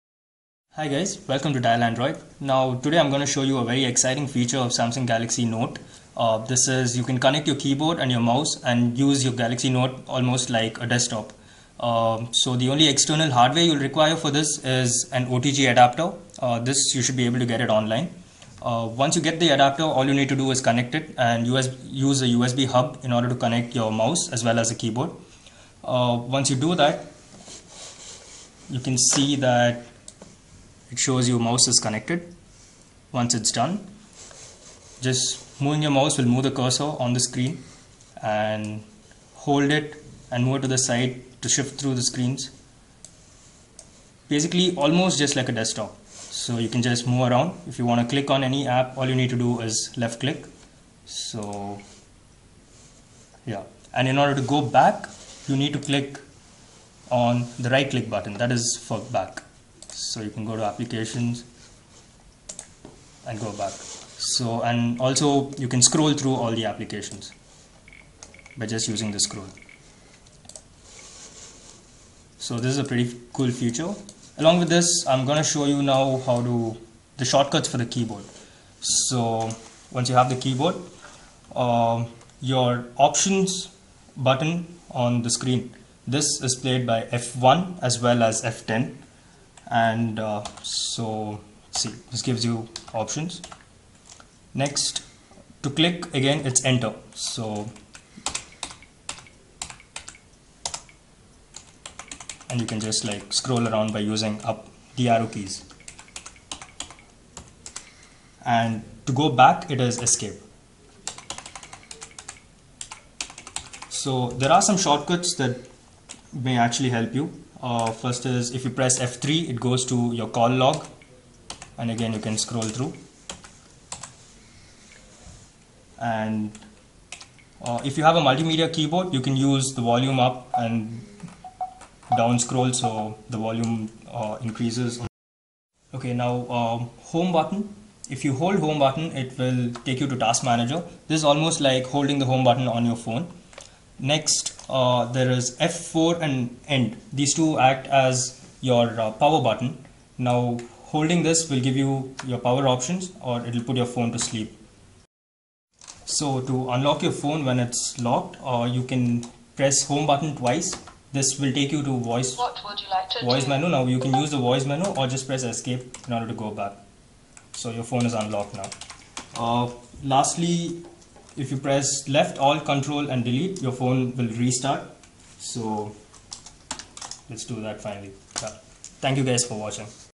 USB on-the-go(OTG) mouse & Keyboard on Samsung Galaxy Note N7000
This video shows connecting mouse and keyboard on Samsung Galaxy Note N7000. This will make a perfect small desktop for you.